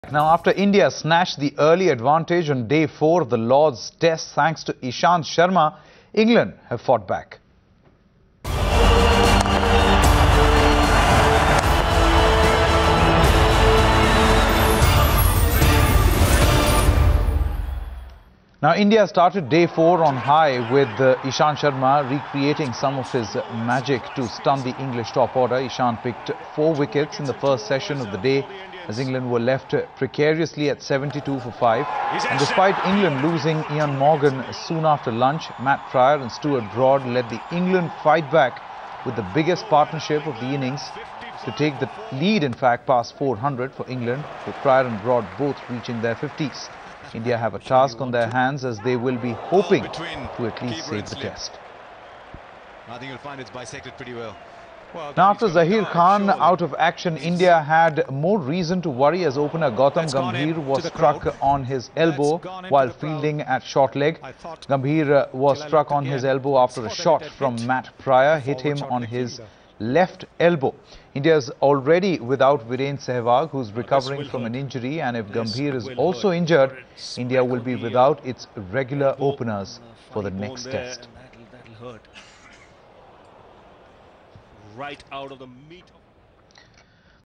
India fight to save the Test
India started day 4 of the first cricket Test at Lord's on high with Ishant Sharma recreating some of his magic to stun the English top order. Ishant picked four wickets in the first session of the day as England were left precariously at 72 for five. And despite England losing Eoin Morgan soon after lunch, Matt Prior and Stuart Broad led the England fightback with the biggest partnership of the innings to take the lead past 350 for England, meaning india will now have to try and save the test at Lord's. Download the Times Now India's Election HQ app and get all the election info at one go. Social Media Links